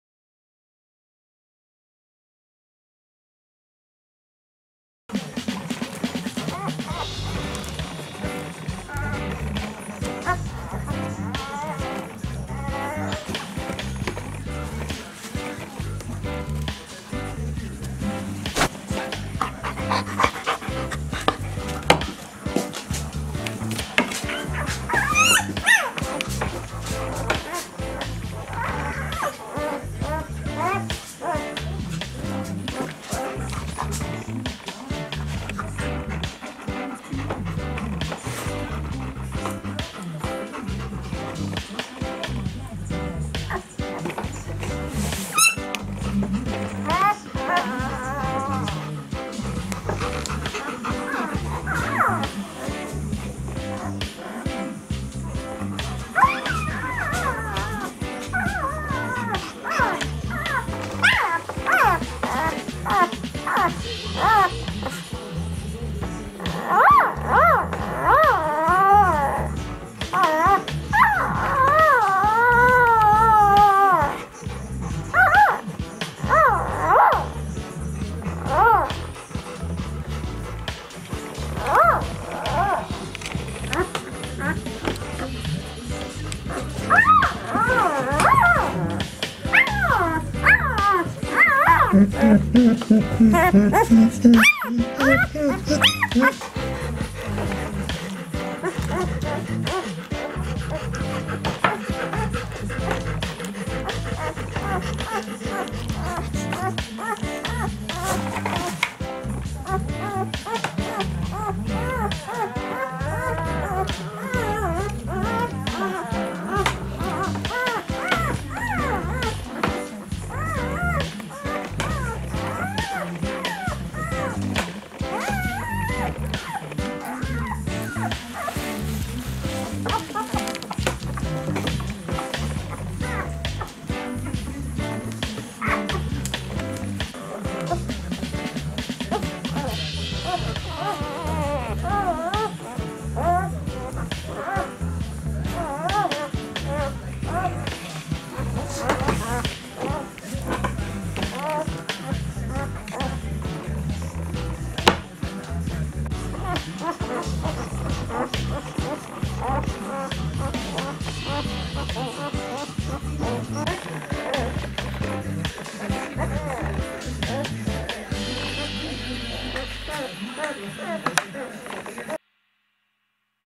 Puppys Chiva xTerror 3 days old
Puppys Chiva xTerror 3 days old all the puppys are growing well today.